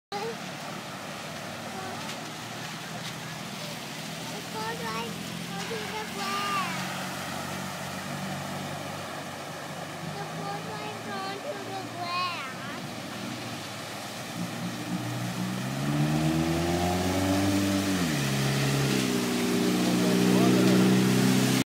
Flooding in Holbrook, MA - August 10, 2012
Route 139 in Holbrook was flooded by a massive rainstorm. It almost never floods.